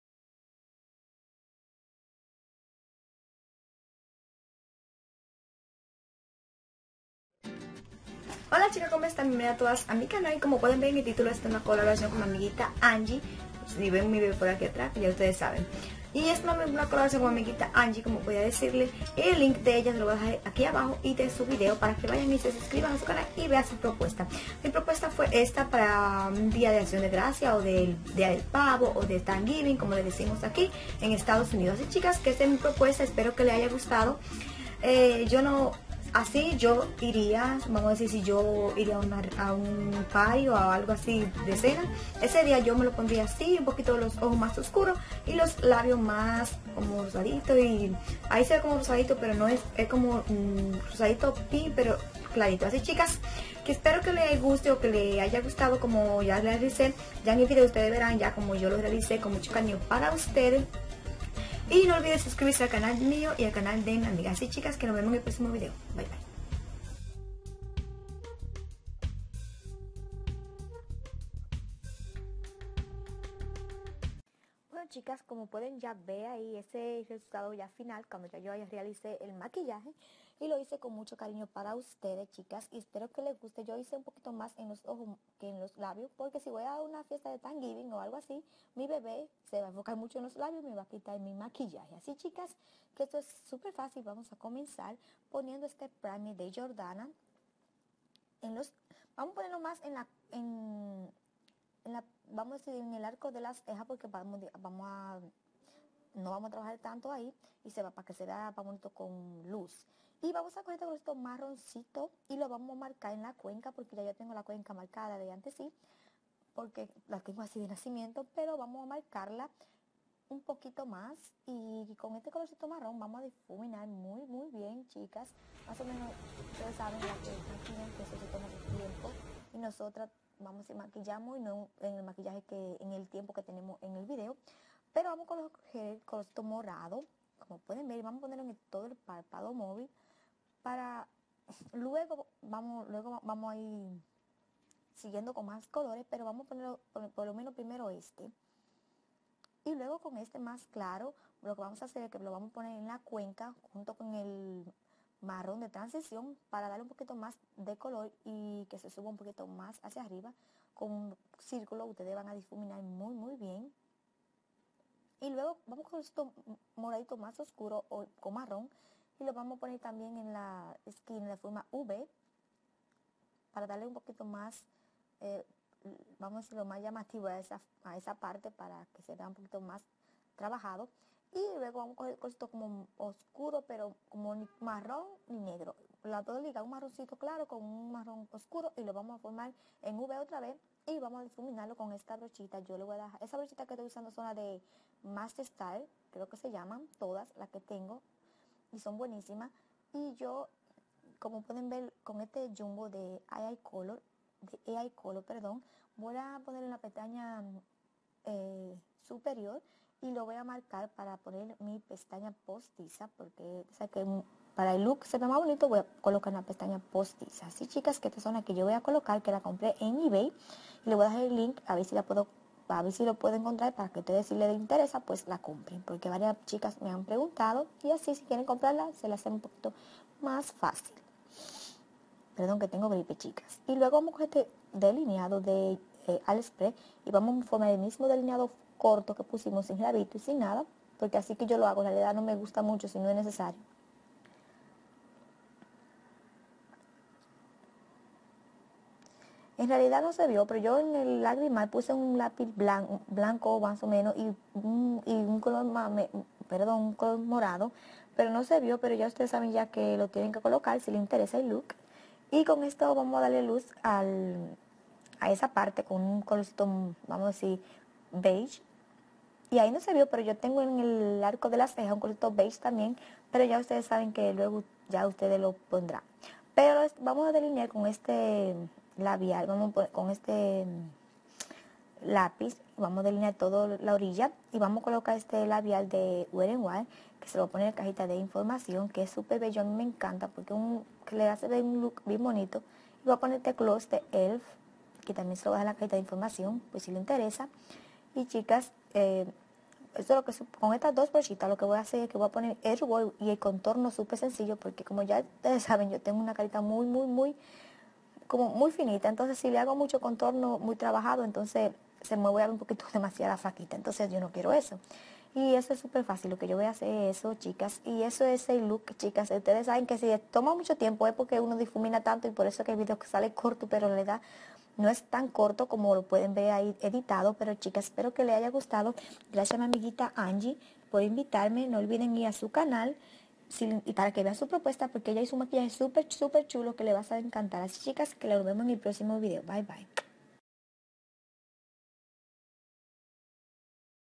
Maquillaje Cena "Accion de Gracias"
chicas espero que mi propuesta les guste ..y gracias a mi amiga anyi por invitarme y tomarme en cuenta.

.BIENVENIDAS.

.sigueme. ­.

facebook. marilenny rodriguez